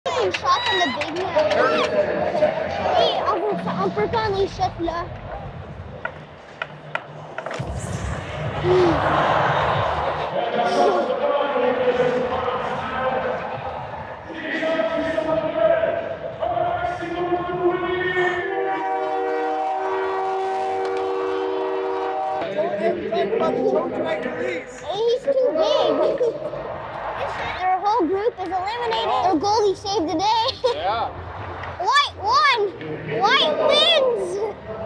Landyn Laberge with Paul Byron's son, Brysen Byron, 2023 NHL Canadiens Skills Competition
Paul Byron's son, Brysen Byron, Shoots on Landyn Laberge LHEQ M17AAA 2023 Canadiens NHL Skills Competition at the Bell Centre.